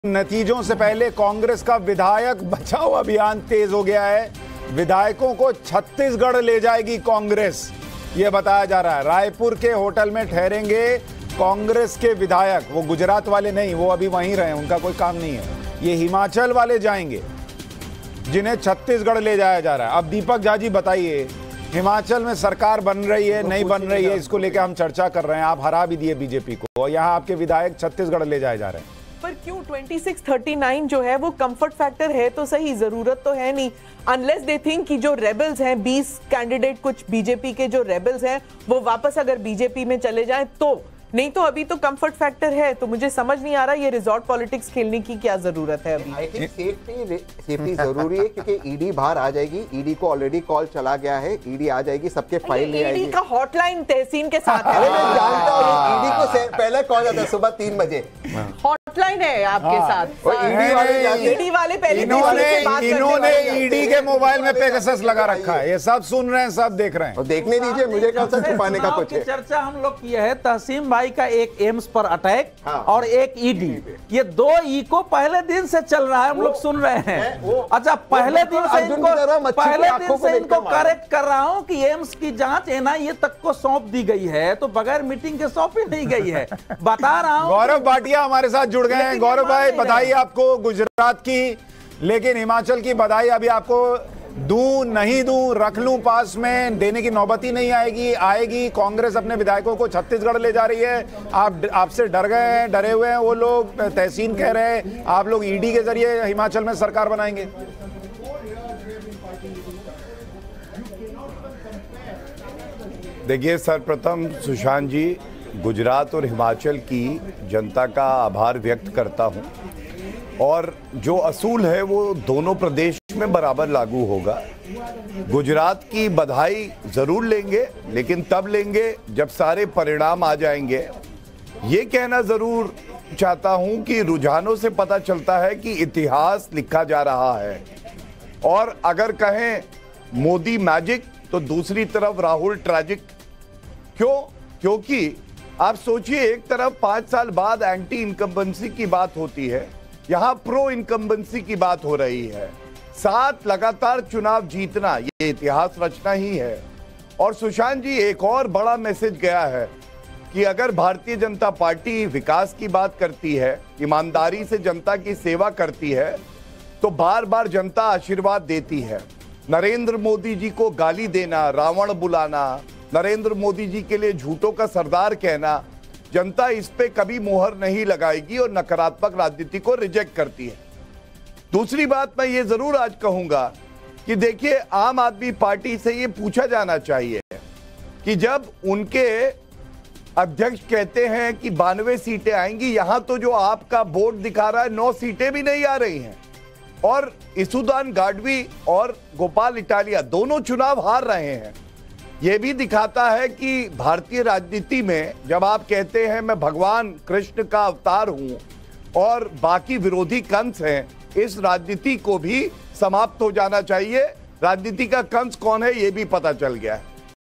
Himachal Election Result | चुनावी नतीजों से पहले Congress का विधायक बचाओ अभियान शुरू
Himachal Pradesh के चुनावी नतीजों में Congress को बढ़त मिलती हुई दिख रही है। इसे लेकर बड़ी खबर है कि कांग्रेस हिमाचल के विधायकों को Chhattisgarh लेकर जाएगी। जहां उन्हें Raipur के होटल में ठहराया जाएगा।